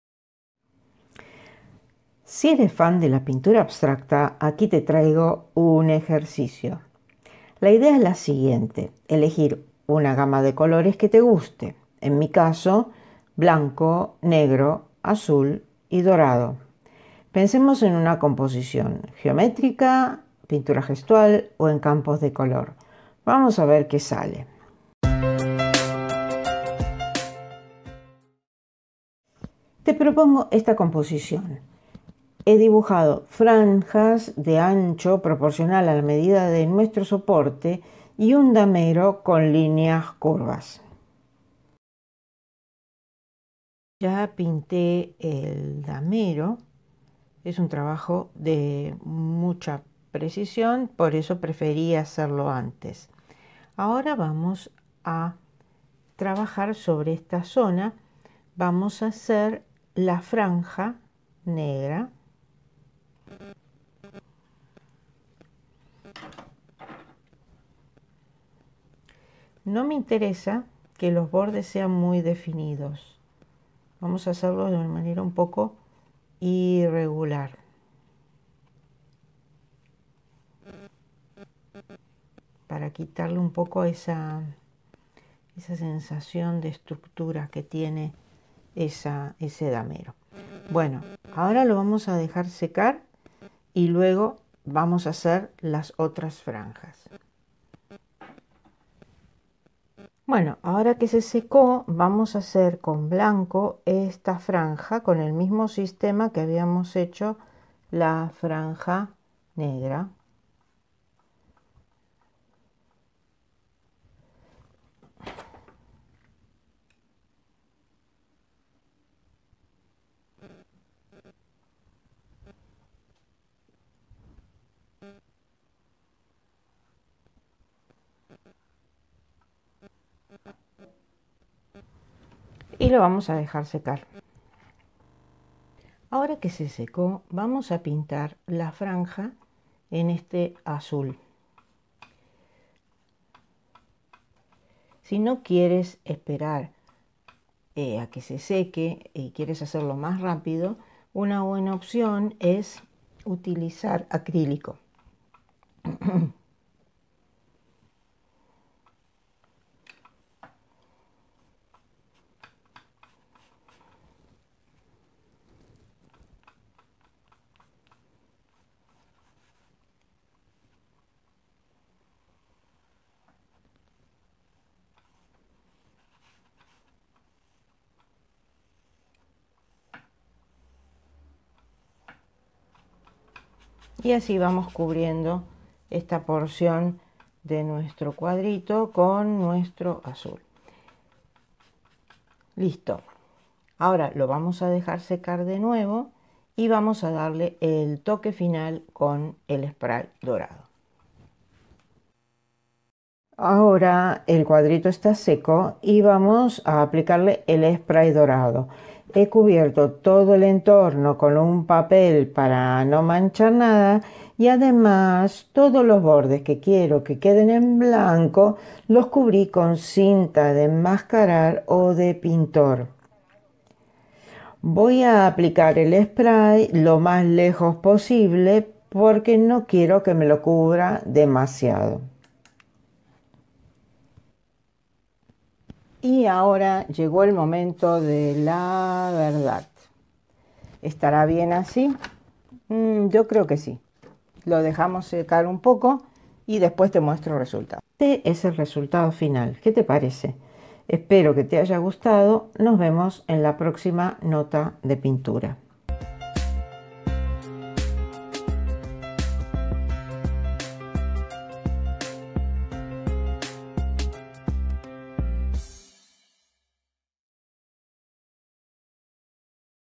3. Notas de Pintura: Un poco de pintura abstracta
Fruto de un pequeño accidente en el taller con un tubo de óleo... Un poco de pintura abstracta de vez en cuando no viene mal, ¿no? La explicación completa en mi blog.